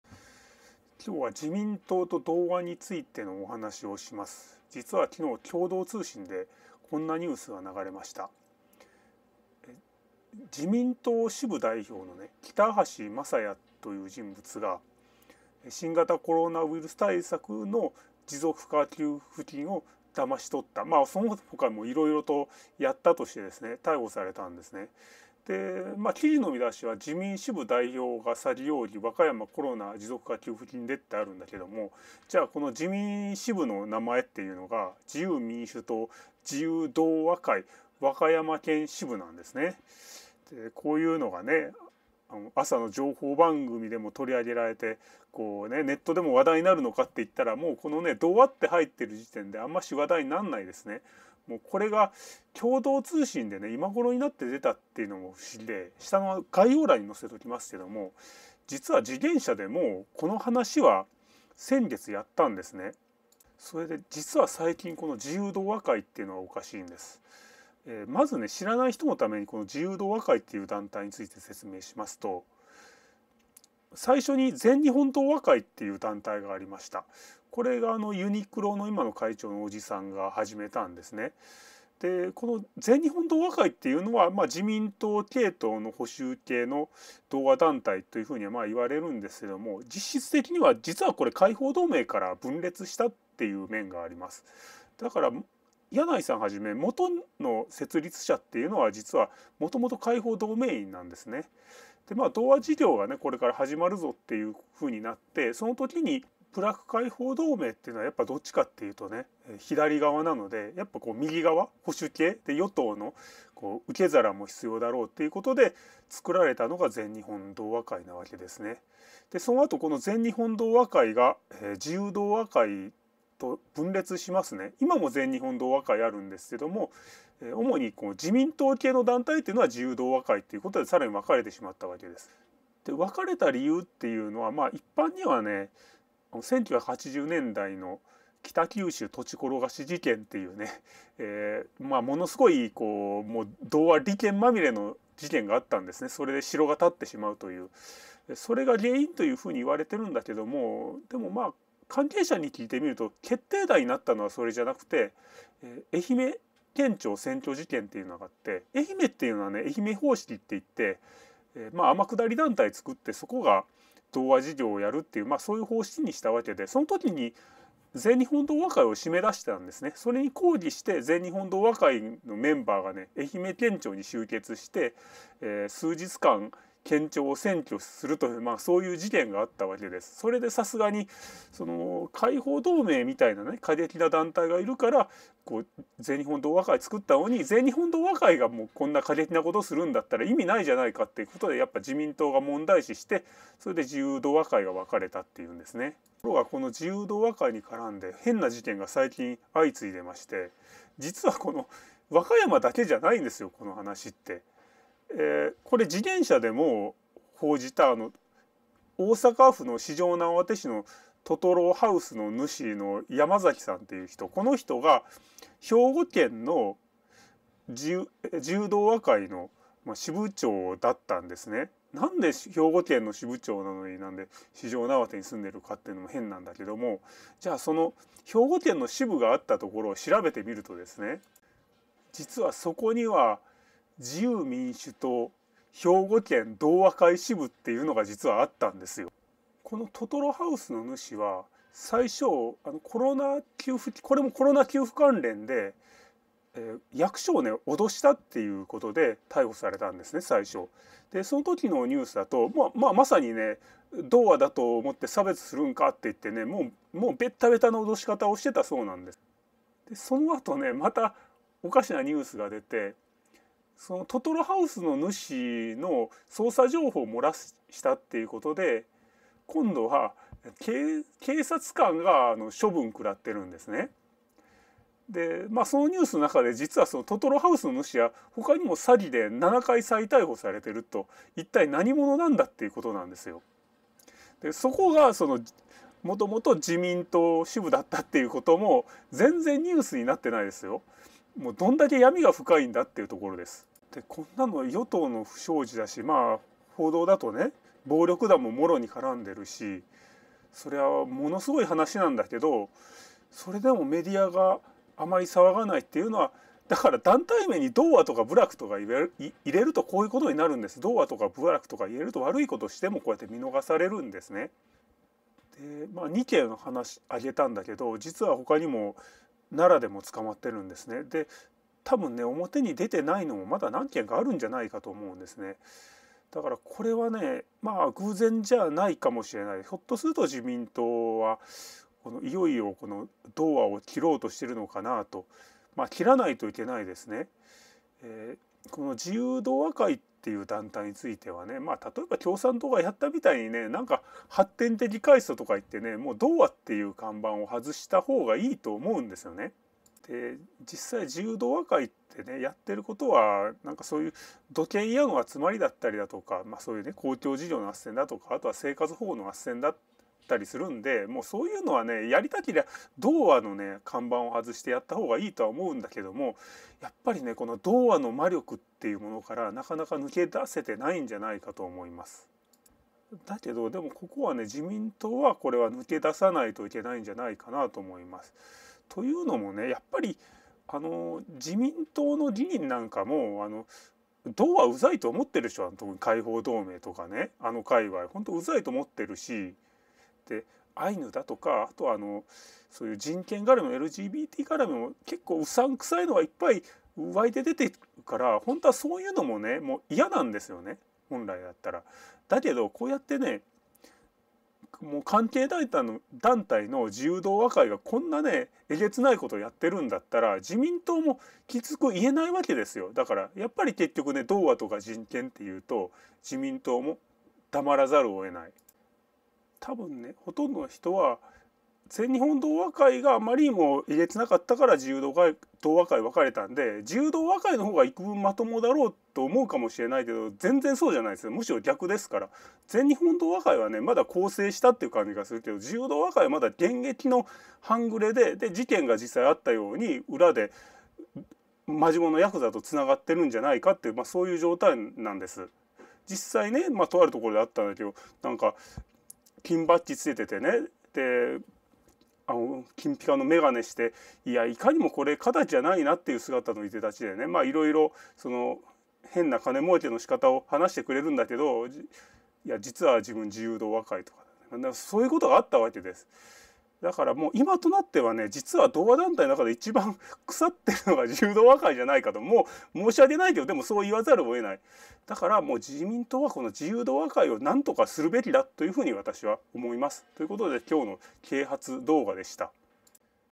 【ミミ】自民党と同和と暴力団【闇深】
最近、自由同和会がらみの怪しげな事件が相次いでいることについて。こちらの記事もどうぞ